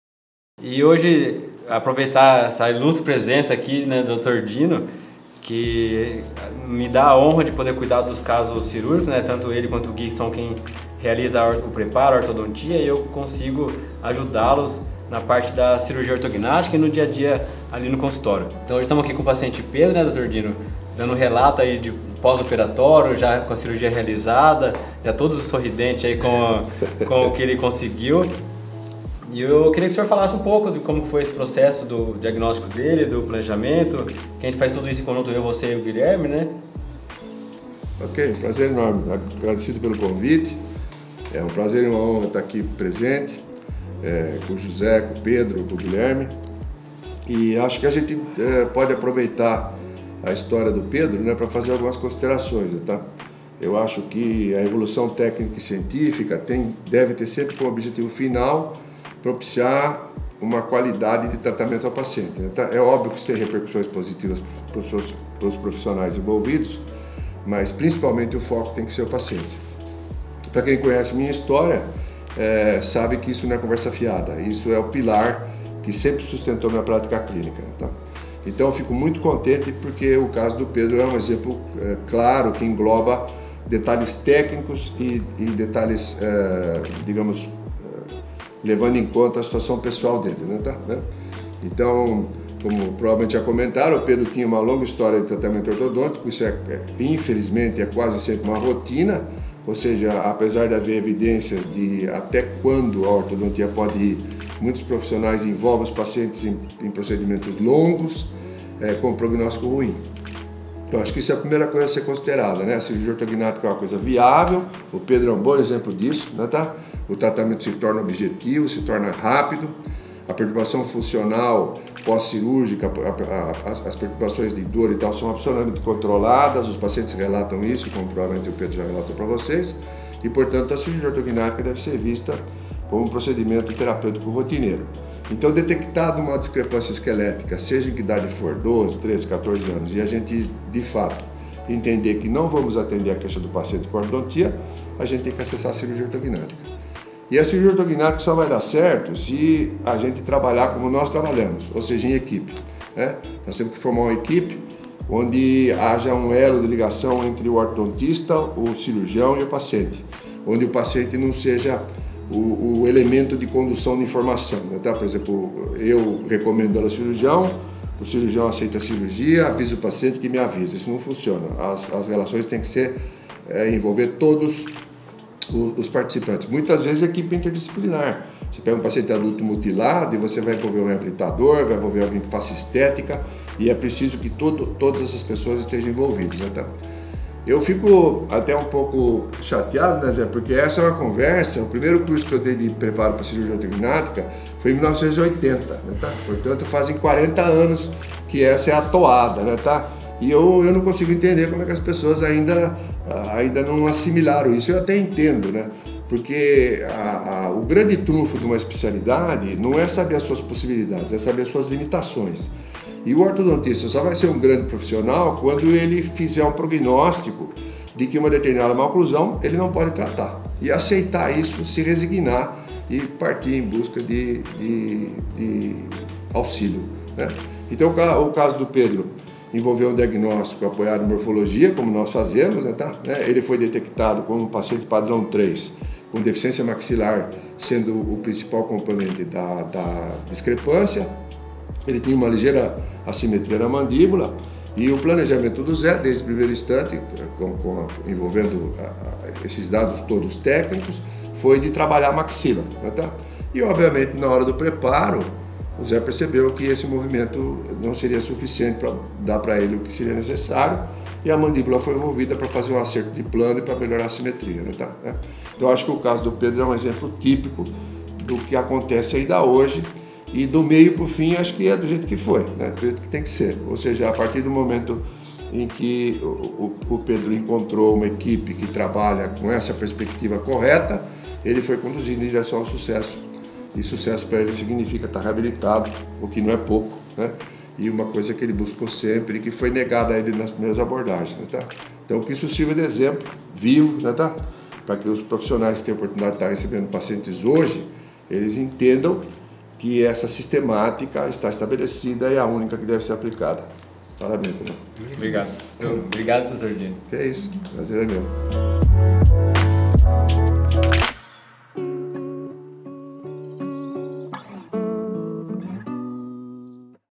CIRURGIA ORTOGNÁTICA: Depoimento Pedro Peruzzo - Resultados e Conquistas | Dr. Zé Bastos
Sempre falei isso e tenho grandes parceiros de trabalho que também pensam assim. E isso me deixa muito satisfeito!
O Dino Capelozza e o Guilherme Capelozza fazem o preparo ortodôntico de muitas cirurgias minhas e o caso do Pedro Peruzzo foi assumido por eles. Paciente padrão 3, planejamos todo o tratamento visando a simetria da maxila e mandíbula. A cirurgia ortognática foi um sucesso e tivemos mais uma grande aula do Dino sobre o preparo ortodôntico para a ortognática.